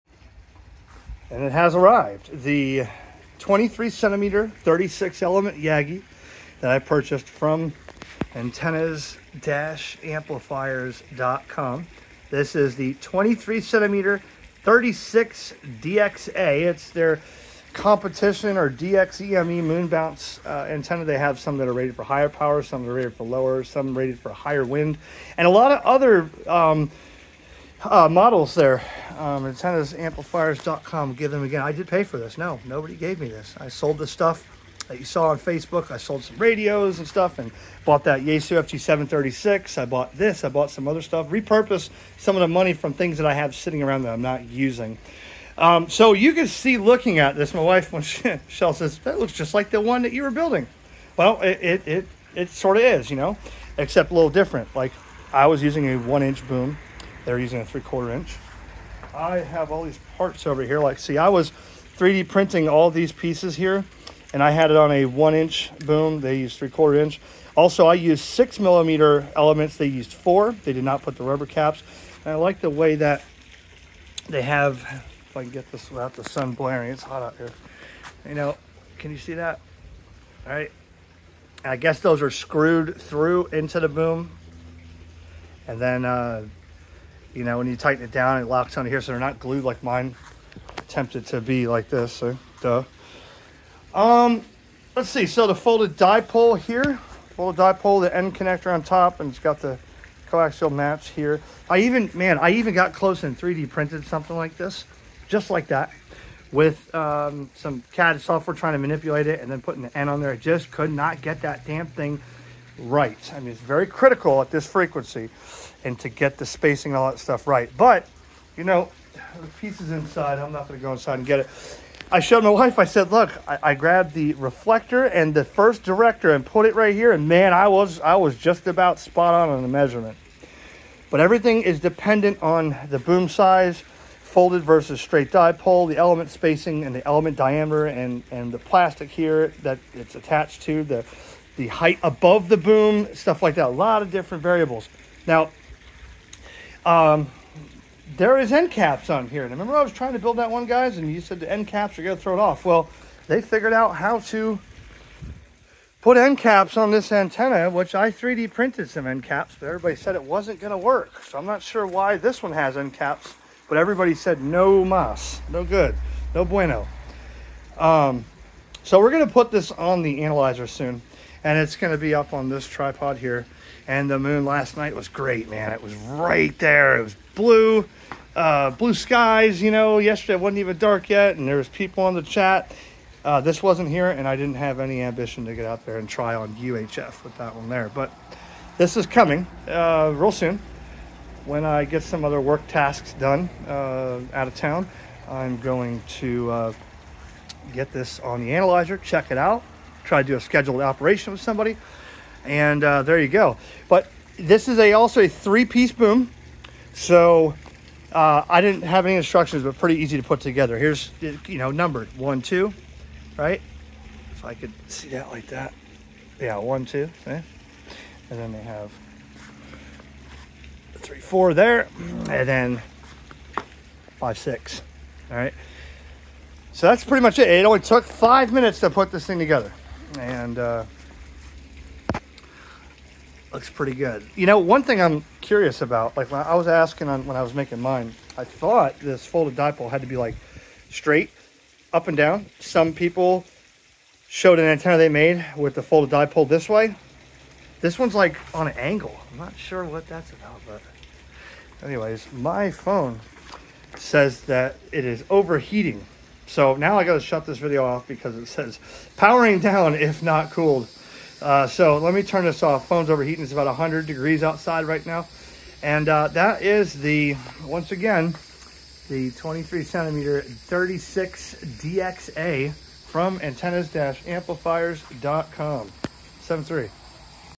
36 Element 1.2GHz Yagi, 23cm36DXA From Antennas-amplifiers.com
it is the 23cm36DXA

I gave up on trying to build one and I went for a commercial version.